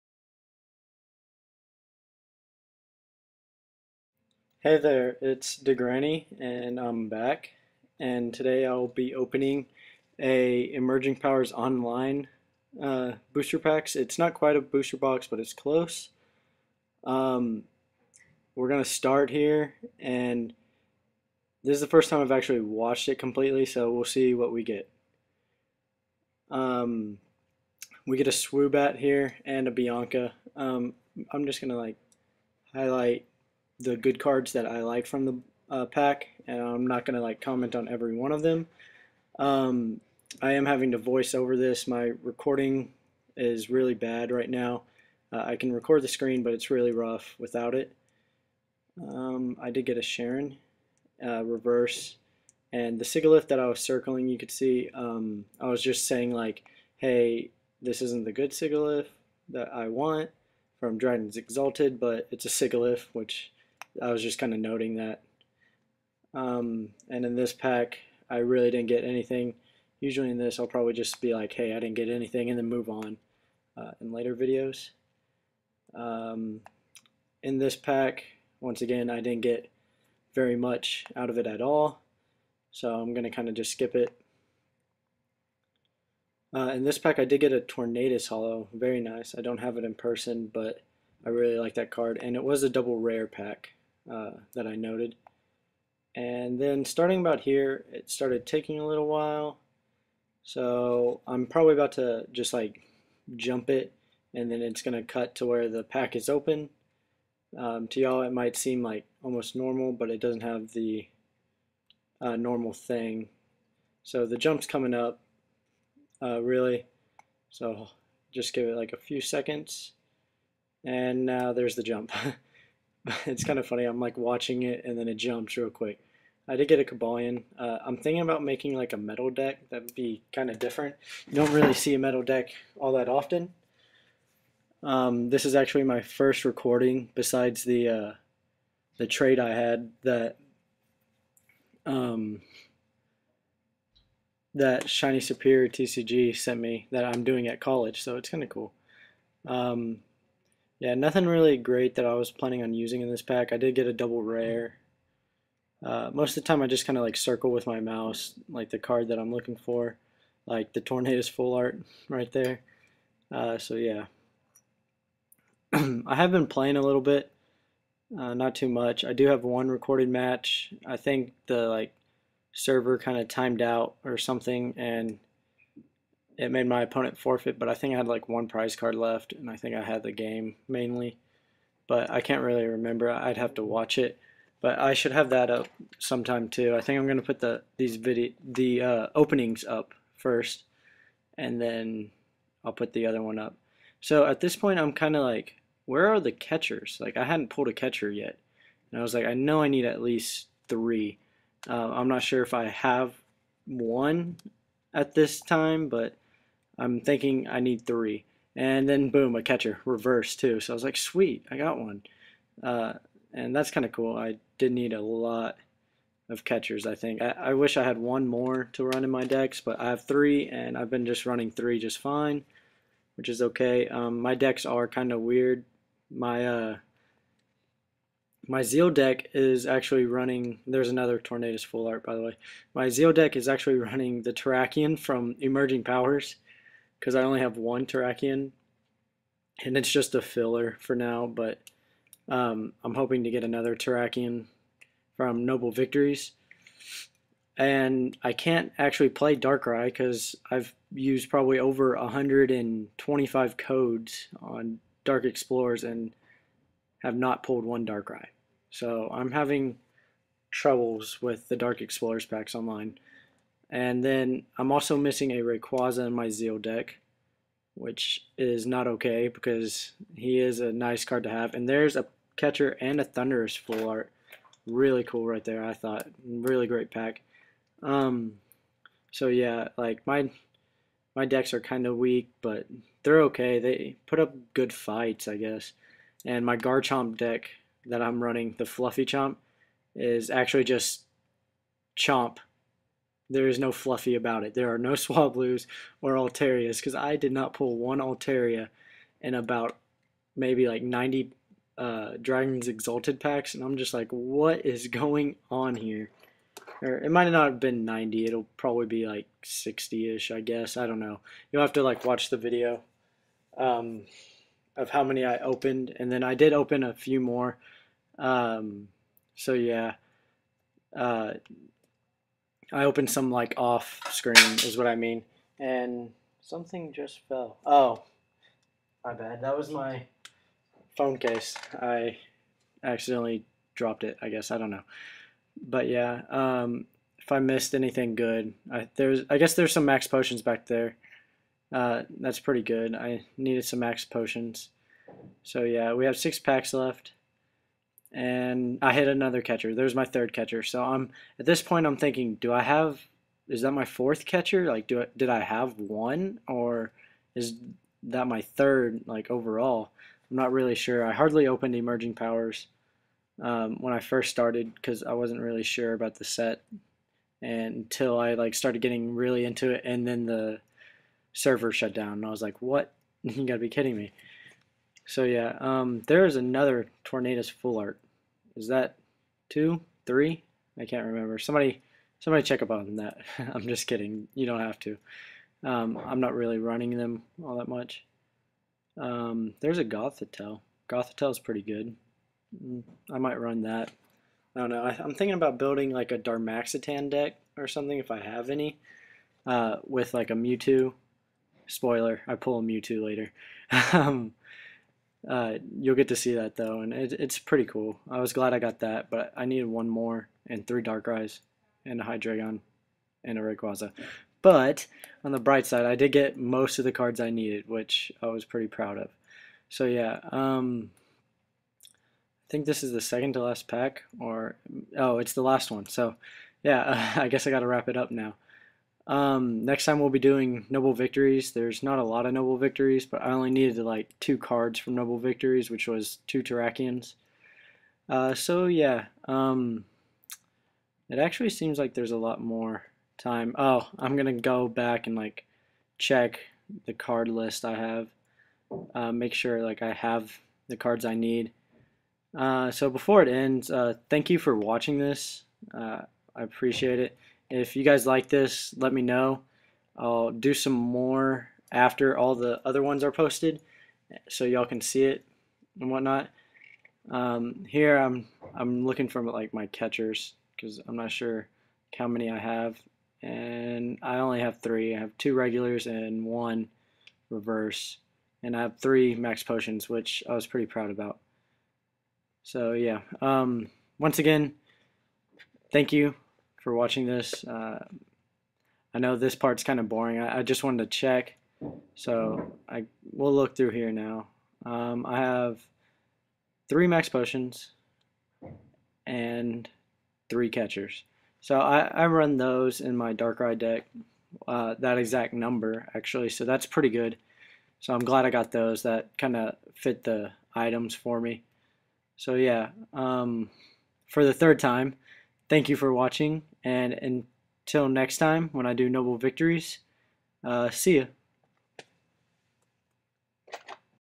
Emerging Powers Online Opening #1
I know I don't go over every card that i opened, mainly just looking for certain cards that I needed!